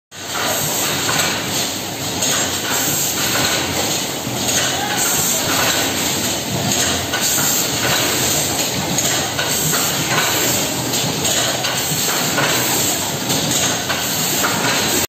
Full Automatic Scale Weighing Hidden Tea Cups Making Machine
this video taken at our clients's factory, they have 12 sets of our full automatic hidden tea cups making machine, and making more than 200,000pcs hidden tea cups every day.
The capacity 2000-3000cups/hour/set, and machine finish electric scale weighing, filling, sealing full automatically.